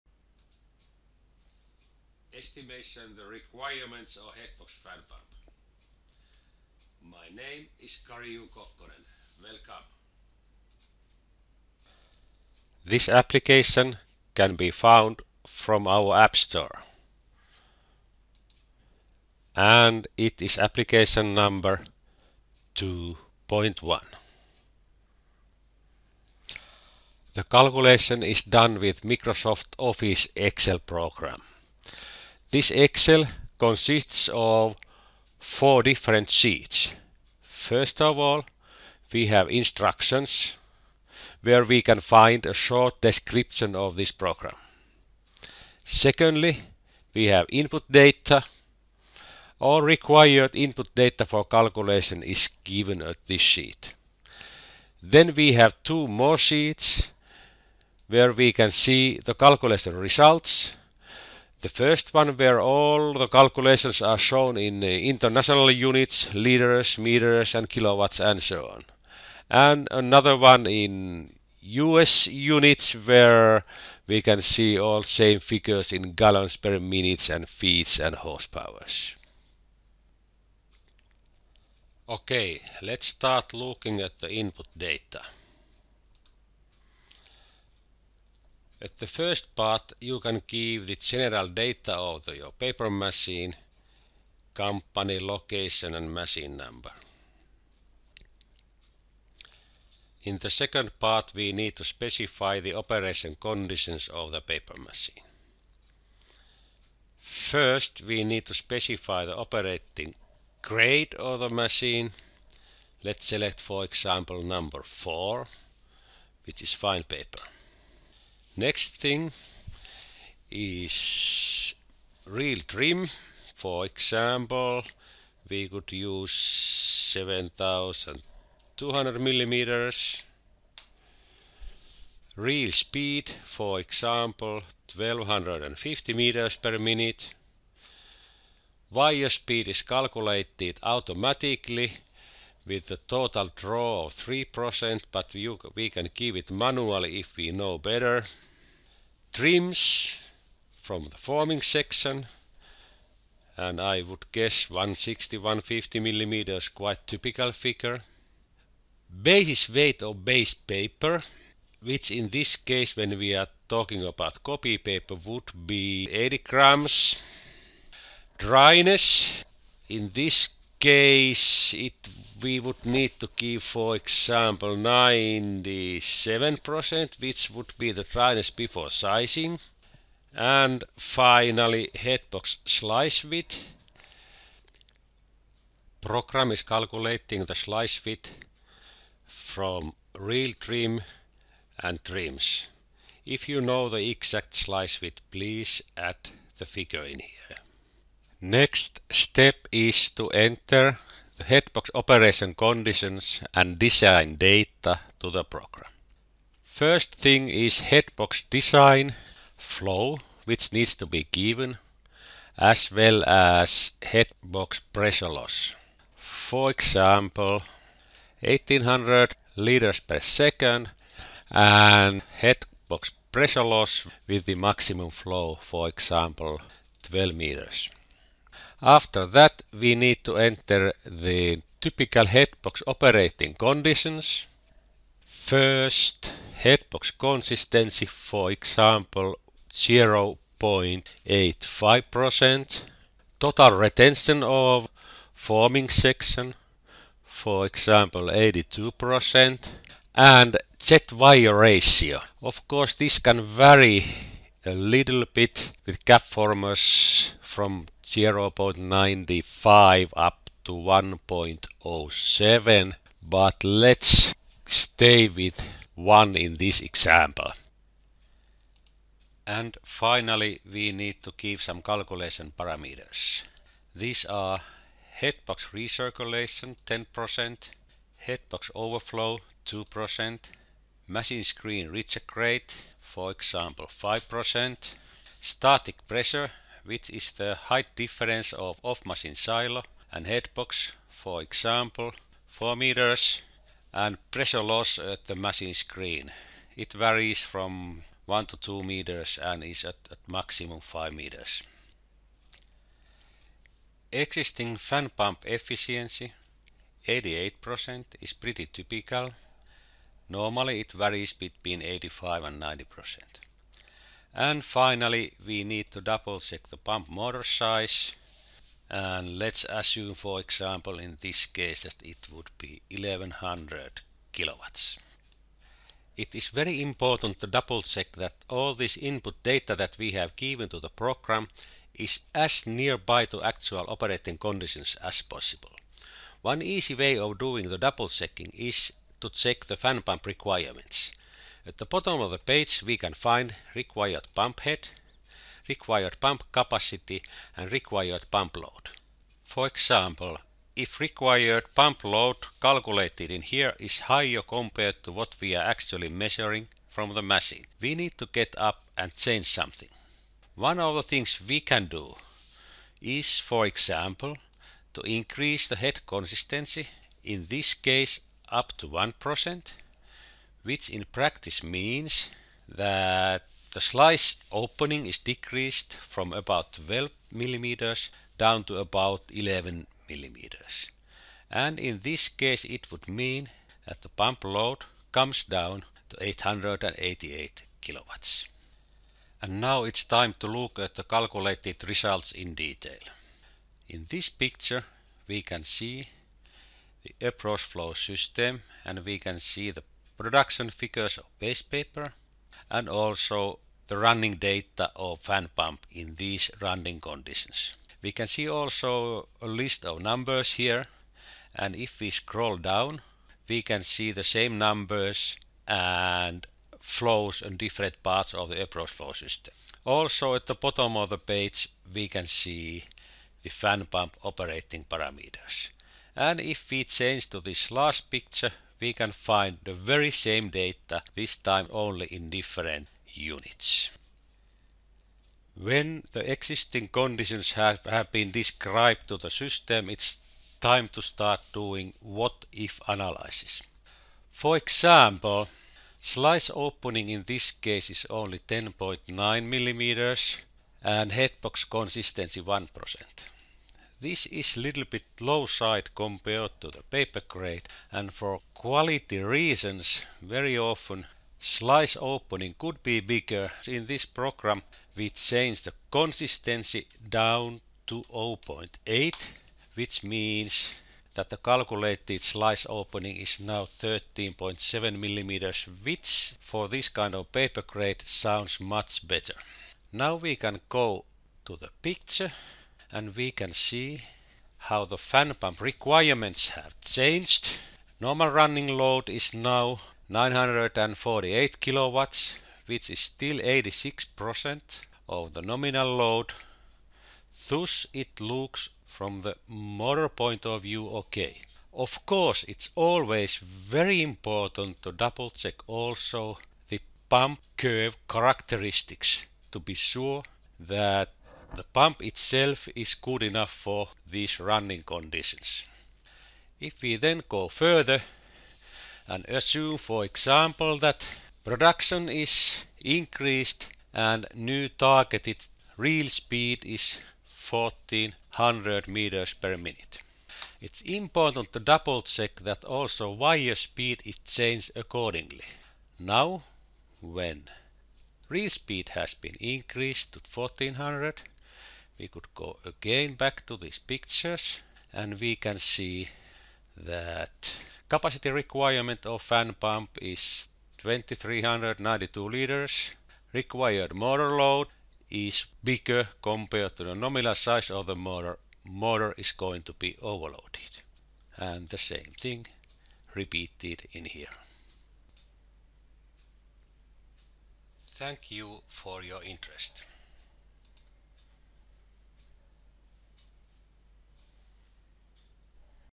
Estimate PM fan pump capacity requirements - Tutorial
Tutorial.
Estimation of capacity requirements of PM fan pump ( = HB feed pump). KGU Engineering.